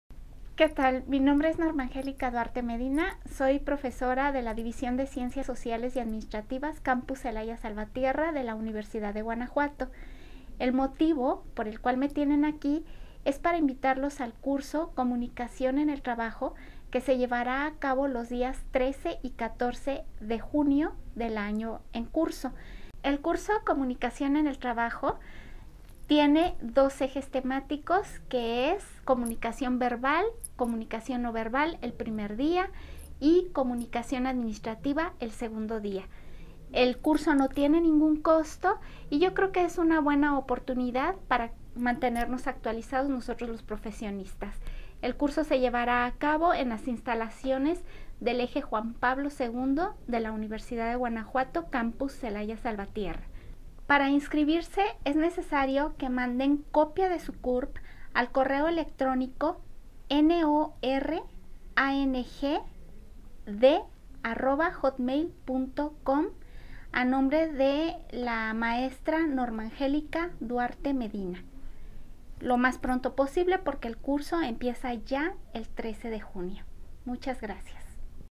Curso Comunicación en el trabajo
Anuncio del curso COMUNICACIÓN EN EL TRABAJO, que imparte la Mtra. Norma Angélica Duarte Medina  en la Sede Juan Pablo II, del Campus Celaya-Salvatierra, el 13 y 14 de junio de 2016, de 8 a 2, Salón 201.